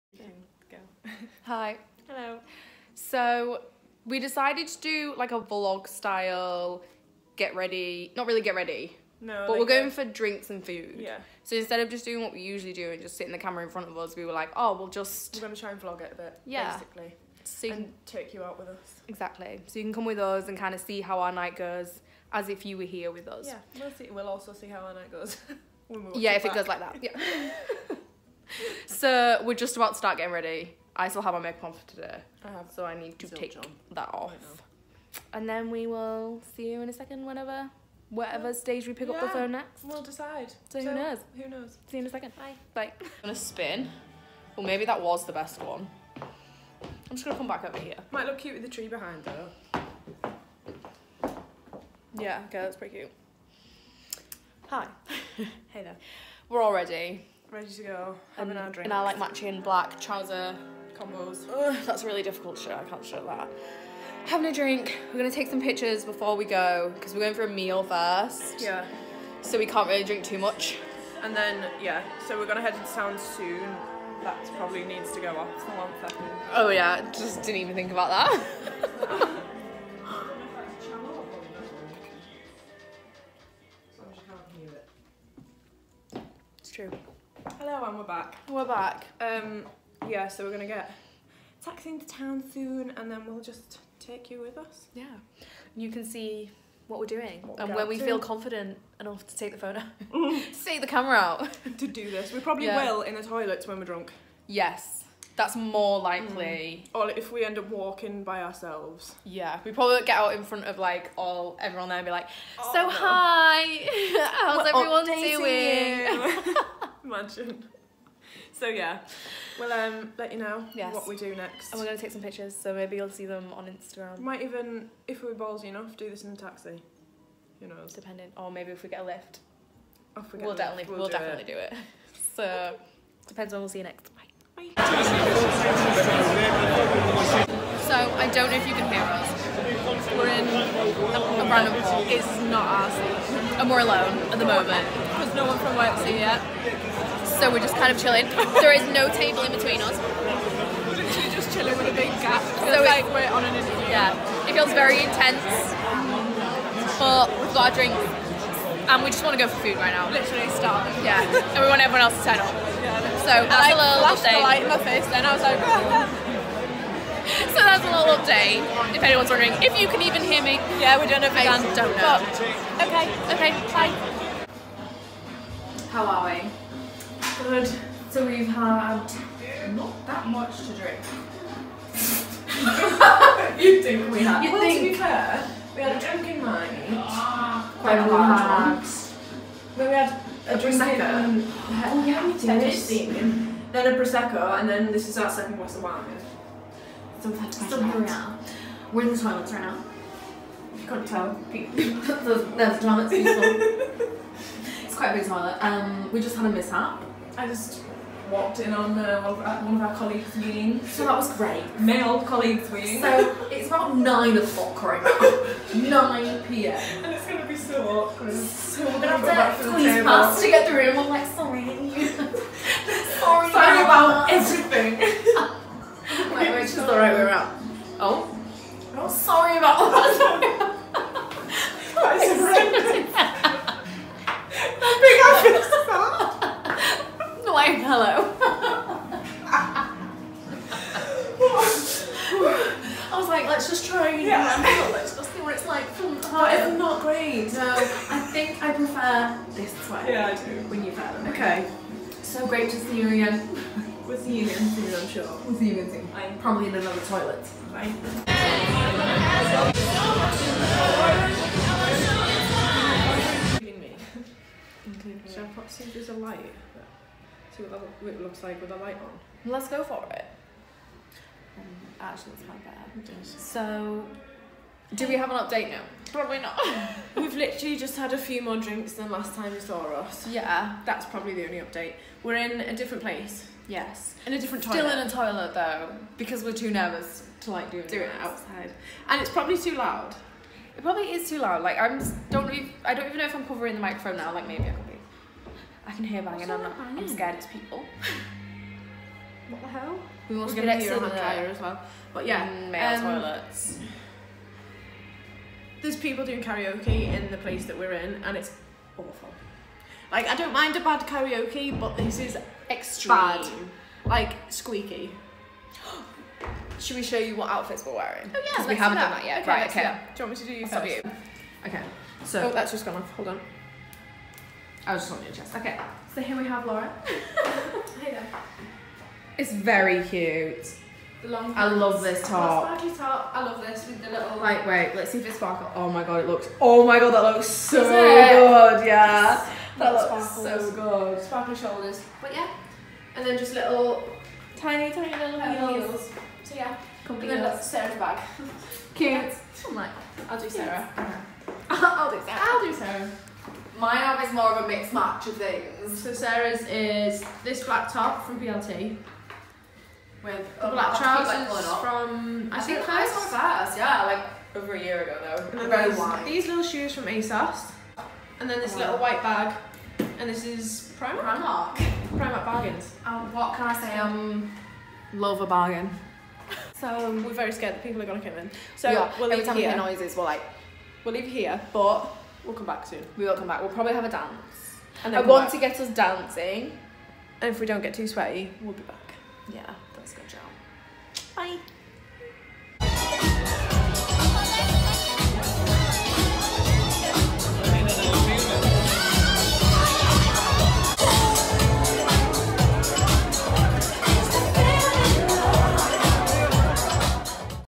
Time for drinks! VLOG Style
We thought we try a little vlog style video of us going for drinks, let us know what you think!! This is just a little video to keep you guys updated,
We had laughs and are messes but we don't have a 'vlog style' camera so sorry about the quality it was recorded on my phone!
sarahbigby
laurakatiea